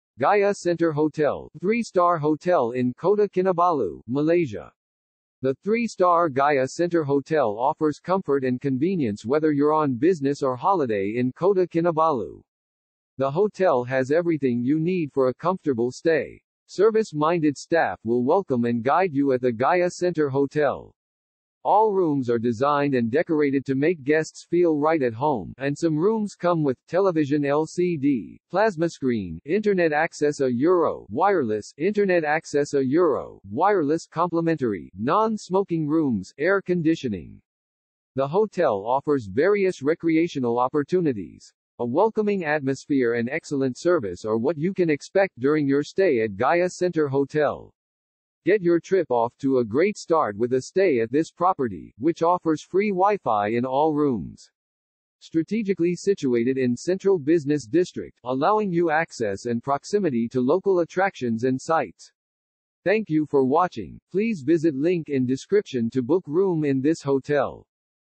Gaya Centre Hotel, 3 Star Hotel in Kota Kinabalu, Malaysia
Gaya Centre Hotel

The 3-star Gaya Centre Hotel offers comfort and convenience whether you're on business or holiday in Kota Kinabalu. The hotel has everything you need for a comfortable stay. Service-minded staff will welcome and guide you at the Gaya Centre Hotel. All rooms are designed and decorated to make guests feel right at home, and some rooms come with television LCD/plasma screen, internet access â€“ wireless, internet access â€“ wireless (complimentary), non smoking rooms, air conditioning. The hotel offers various recreational opportunities. A welcoming atmosphere and excellent service are what you can expect during your stay at Gaya Centre Hotel.